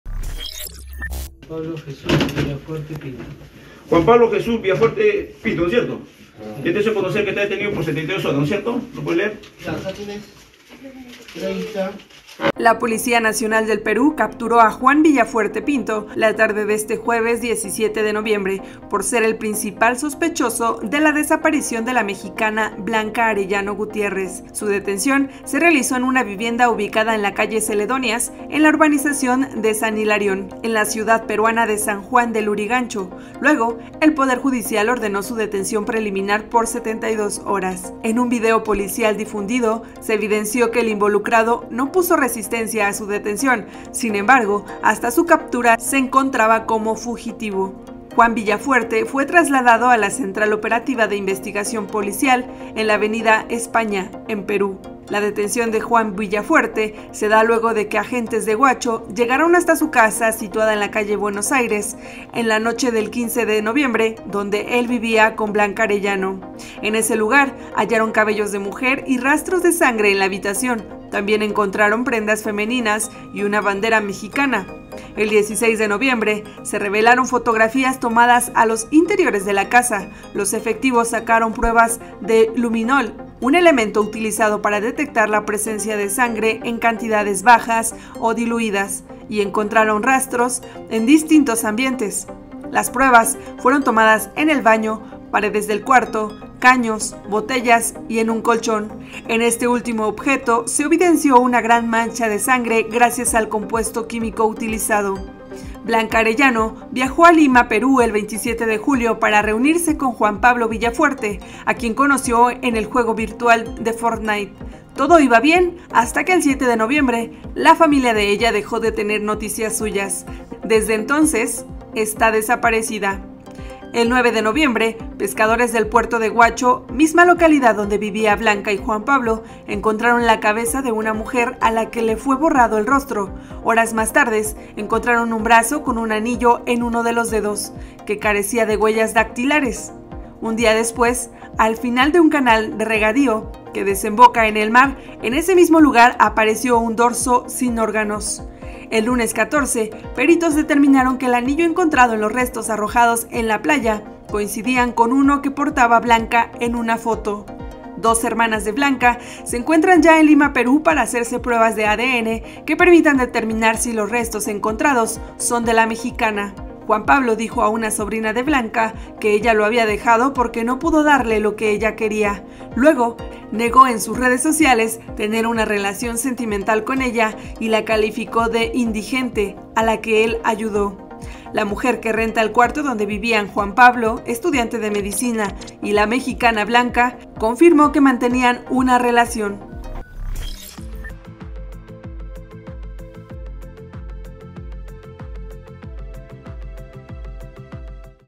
Capturan a presunto feminicida de la mexicana Blanca Arellano en Perú; era su pareja sentimental
El sospechoso Juan Villafuerte se entregó sin resistencia a la policía, que lo investiga por feminicidio y tráfico de órganos